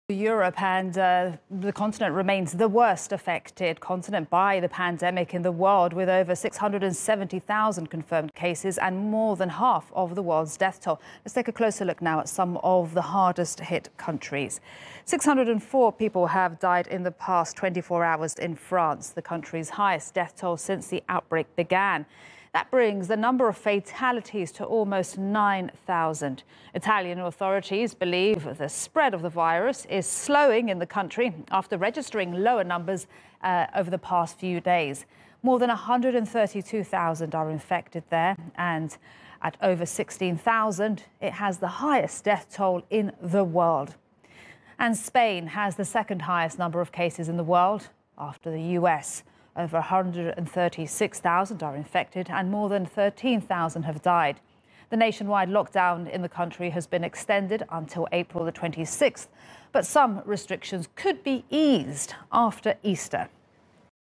Latest on Europe, the worst affected continent - April 7th
Europe is the most affected continent of the coronavirus pandemic and has more than half of the deaths in the world. These are latest in some of the countries in Europe.